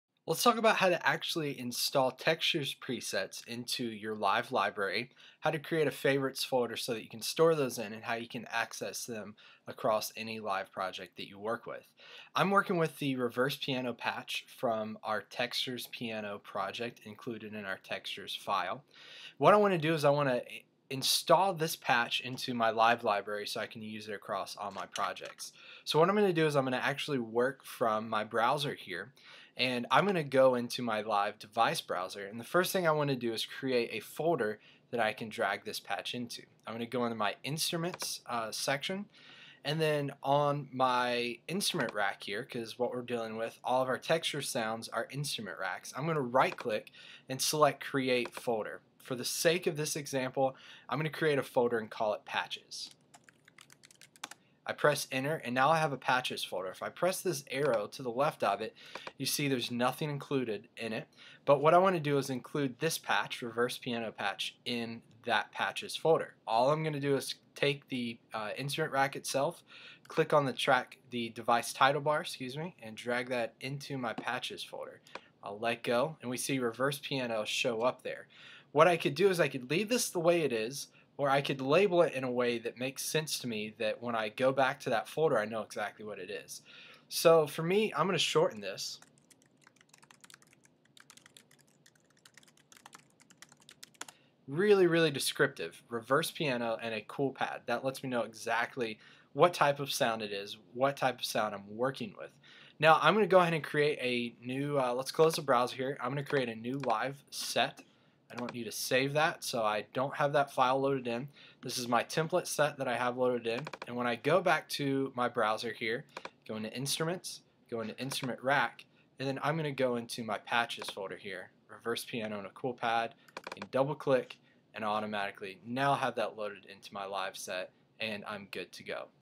Saving Textures Presets into Live's Library
How to save and rename your favorite Textures Patches into Ableton Live